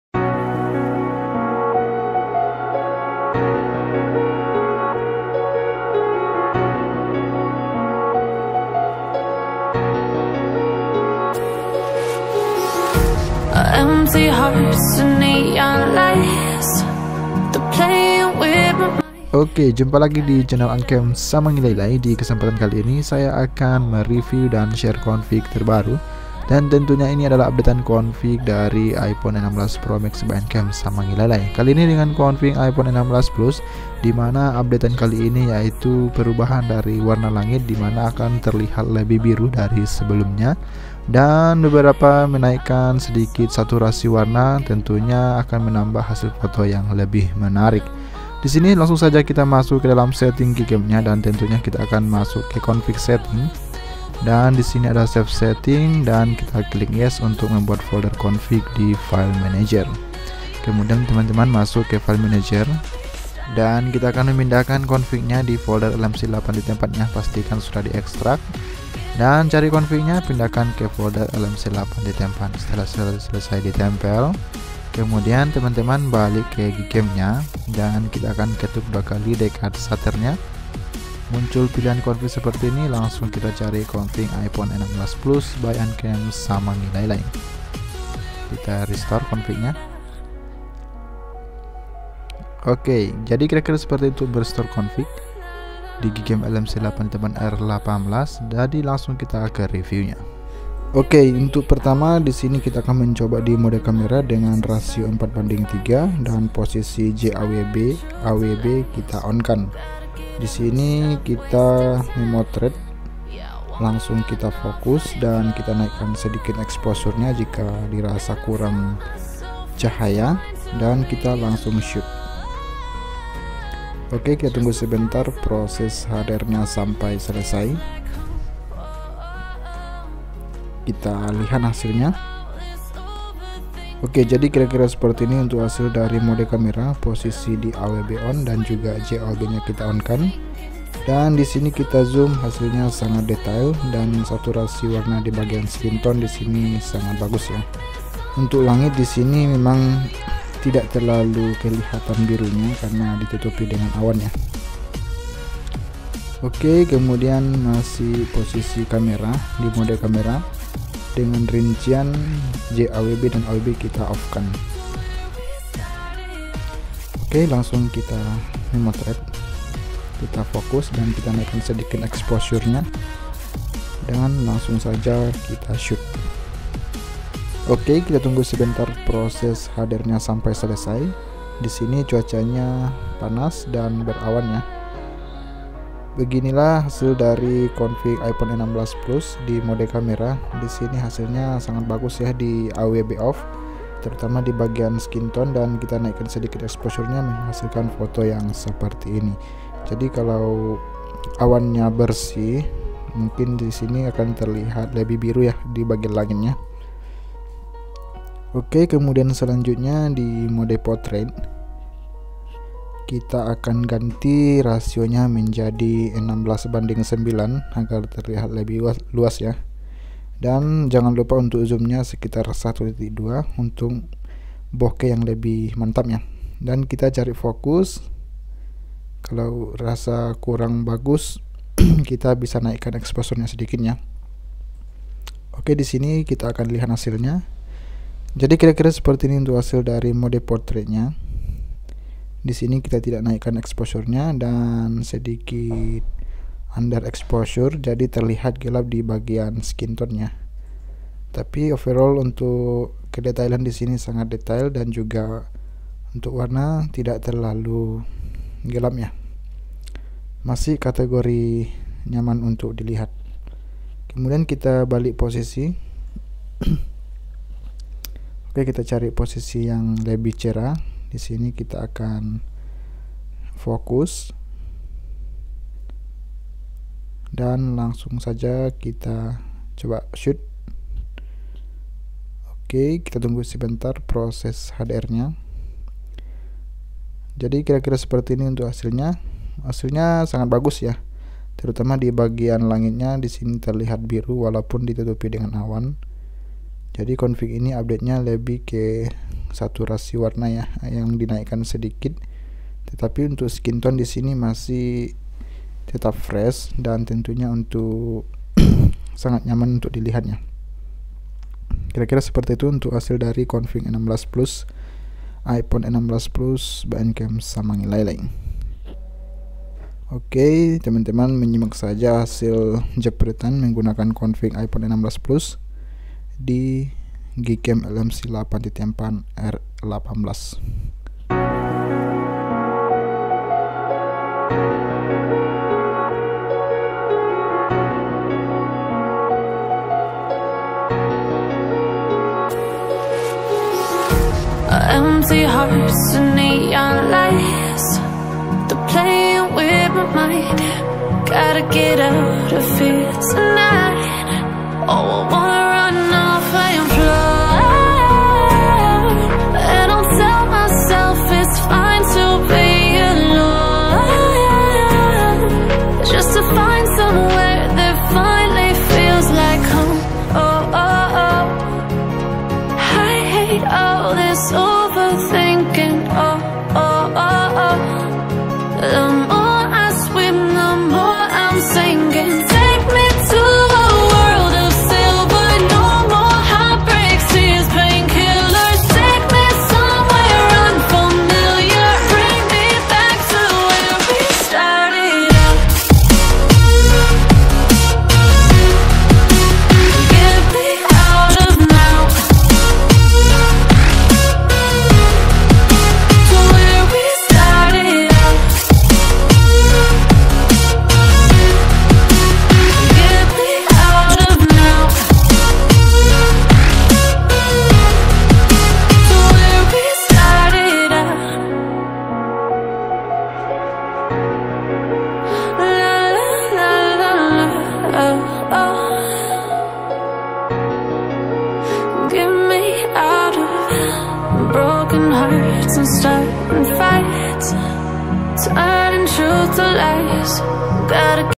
Update Config ‼️ Gcam LMC 8.4 R18 Config iPhone 16 Plus || Hasil Foto Semakin Berwarna 🎉
Review dan Share Gcam LMC 8.4 R18 Config iPhone 16 Plus
Config ini Updatean Config Sebelumnya ‼️

Test Device : 📱 Redmi Note 9 Pro

⚠️ No Password

Jangan Lupa diekstrak dulu ya Confignya ✅